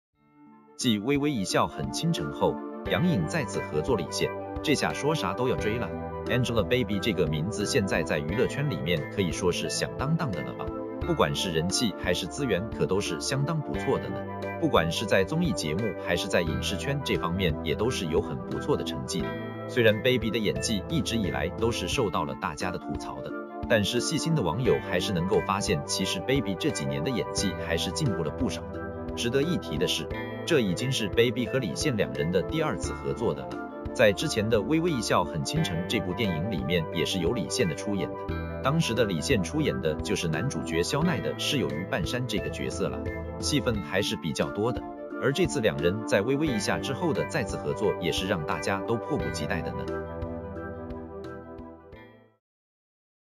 继《微微一笑很倾城》后,杨颖再次合作李现,这下说啥都要追啦
➤Thanks收看！